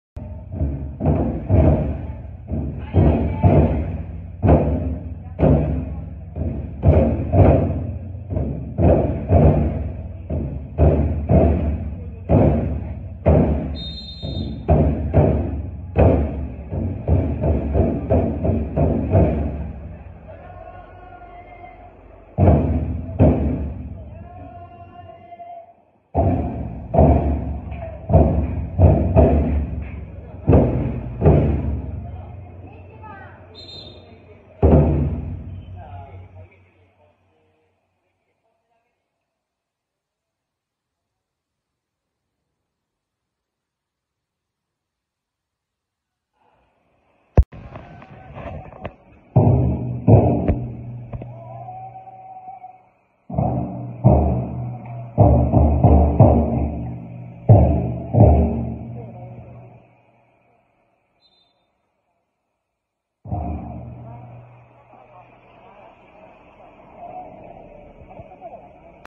令和4年 出島 ふとん太鼓 太鼓の練習
ふとん太鼓 出島 太鼓の練習
船待神社秋季例大祭の本番9月15日16日17日に向けて練習です

一ファンとして 皆様方の素晴らしい勇姿を拝見させて頂きます
3年分の熱い思いを担いでください 事故の内容に楽しみにしています